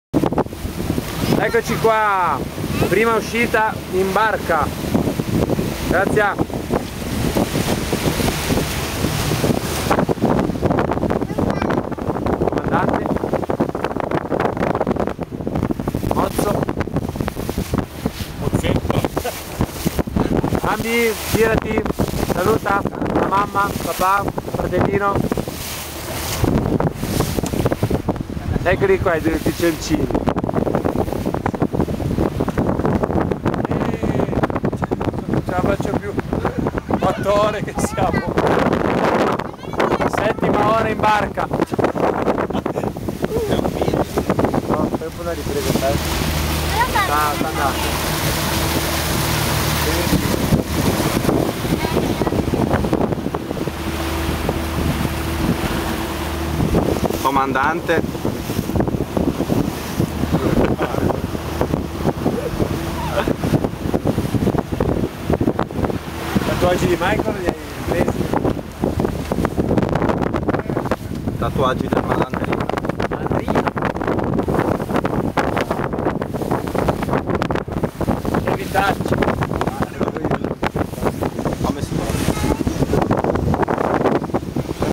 Sessa oyster e famiglia
Prima uscita verso Crevari, a bordo il "comandante" Rino il "mozzo" Eliseo ,Maico, Bambi, Graziano, Zio Seba e il mitico Dennis